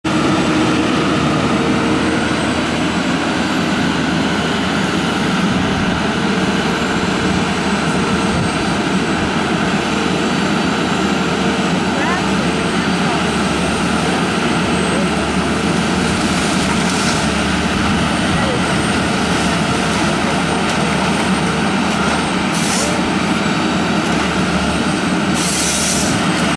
Eucalyptus Harvest at International Paper site
Forestry study tour in Brazil's southern most states.